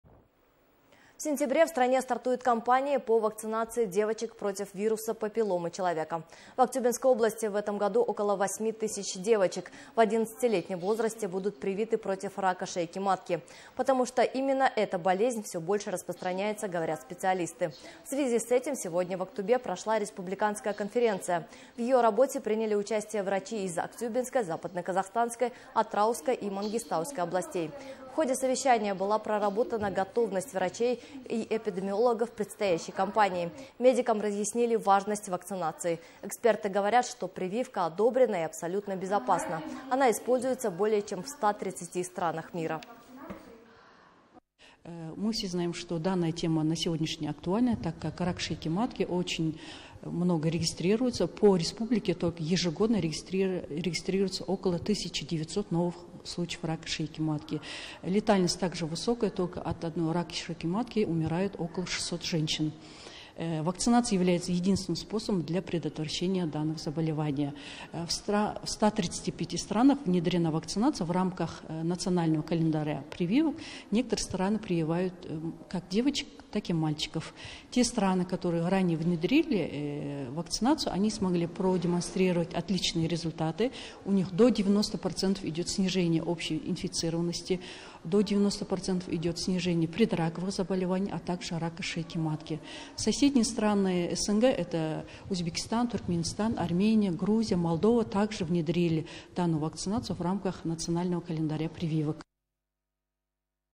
В сентябре стартует вакцинация девочек от вируса папилломы
В сентябре в стране стартует кампания по вакцинации девочек против вируса папилломы человека. В Актюбинской области в этом году около 8 тысяч девочек в 11-летнем возрасте будут привиты против рака шейки матки. Потому что именно эта болезнь всё больше распространяется, - говорят специалисты.

В связи с этим сегодня в Актобе прошла республиканская конференция. В её работе приняли участие врачи из Актюбинской, Западно-Казахстанской, Атырауской и Мангистауской областей. В ходе совещания была проработана готовность врачей-эпидемиологов к предстоящей кампании. Медикам разъяснили важность вакцинации.

 Эксперты говорят, что прививка одобрена и абсолютно безопасна. Она используется более чем в ста тридцати странах мира.